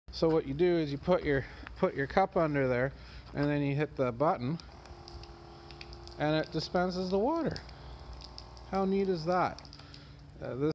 Amazon rechargeable water bottle pump dispenser
Here is an interesting device we got through Amazon, it's a rechargeable pump that you can put on top of a water bottle to dispense the water. You don't need a fancy stand or cooler unit to hold the bottle anymore this will be great for camping !